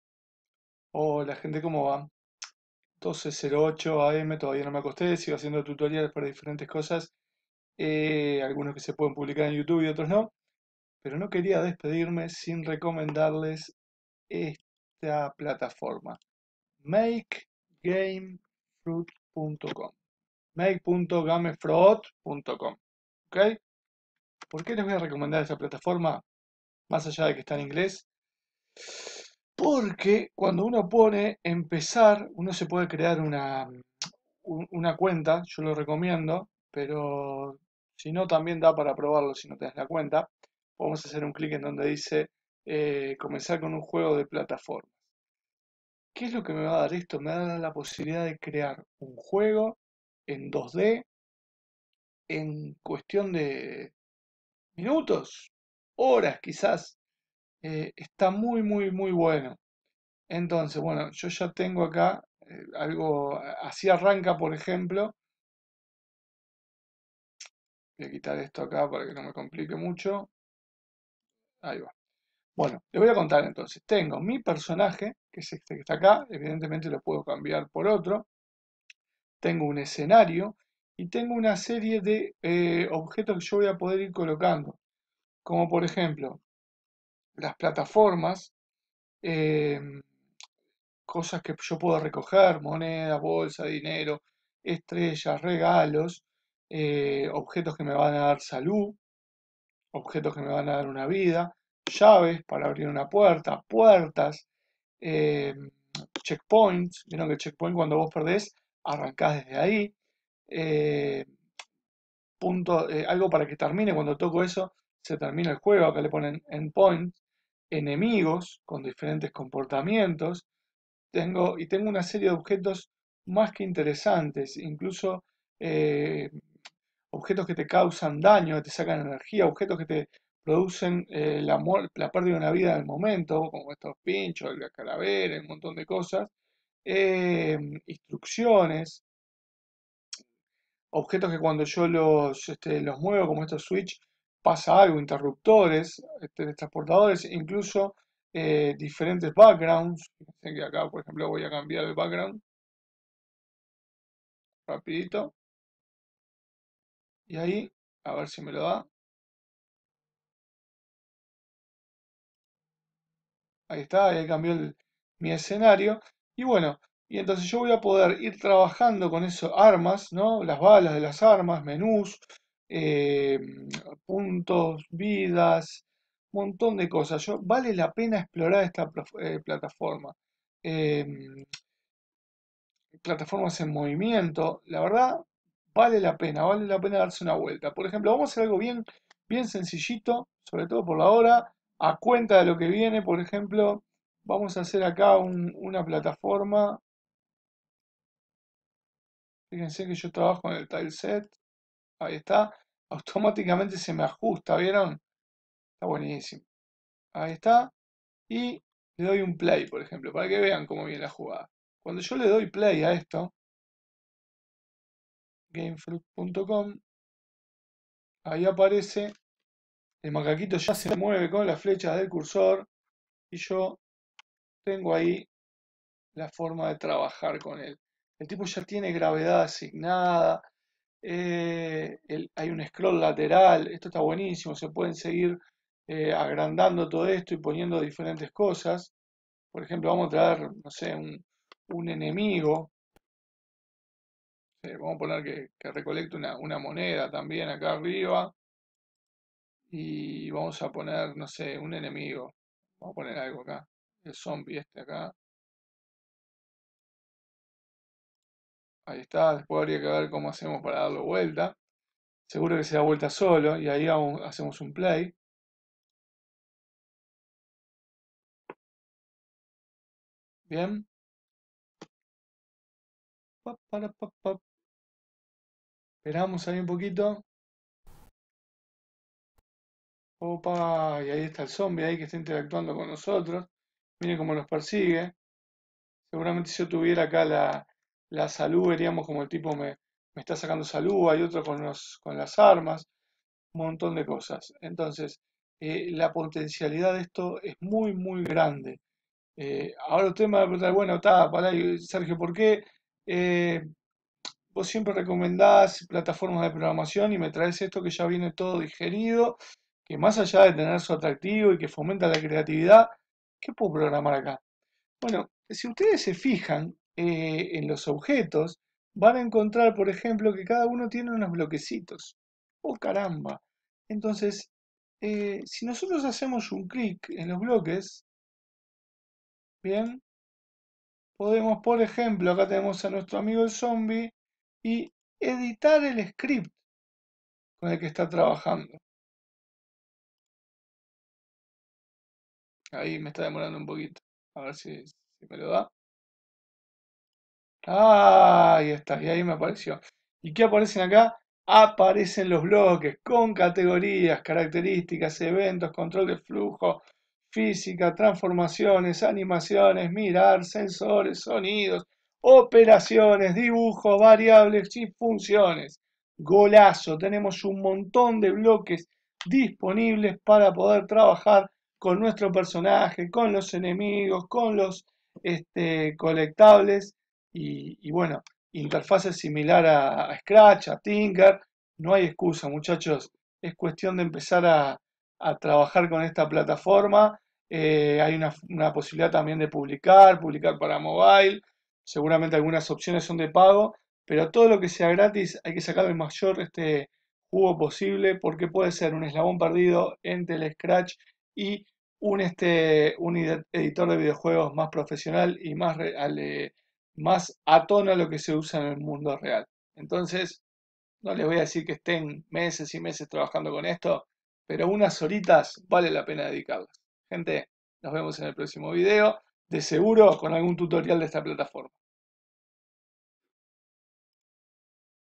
Crea juegos en poco tiempo con gamefroot
es un editor de videojuegos que parece sencillo pero tiene mucho potencial puesto que combina sprites con scripts ya incorporados como un editor de bloques simil Scratch para programar más detalles o ajustar lo ya programado